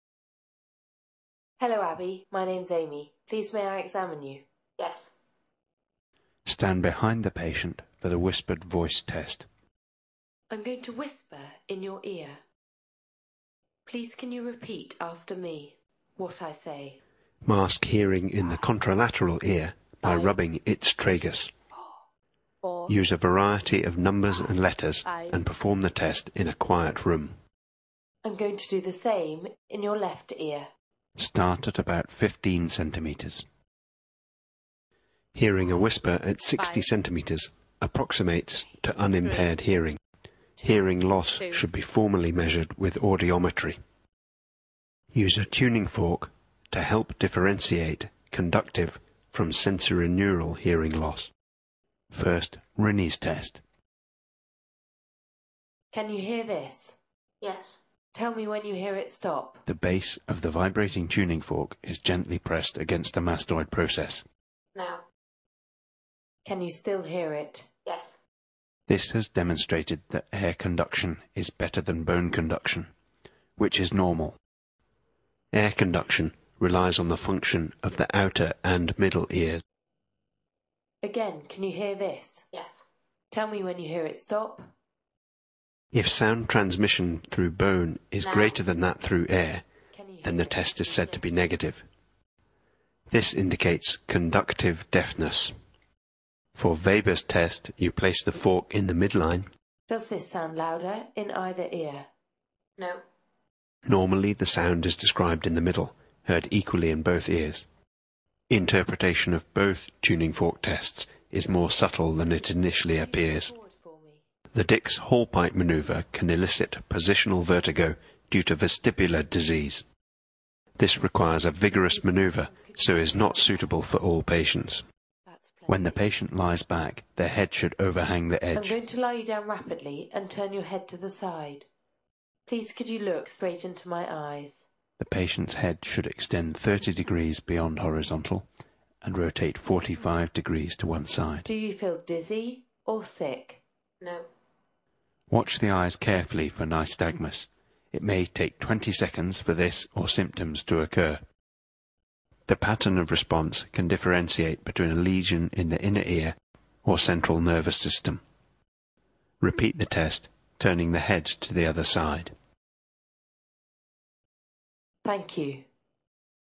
Vestibulocochlear (7th) Cranial Nerve Exam
7th Cranial nerve assessment using whisper test, Rhinne's and Webber's test with 512Hz tuning fork.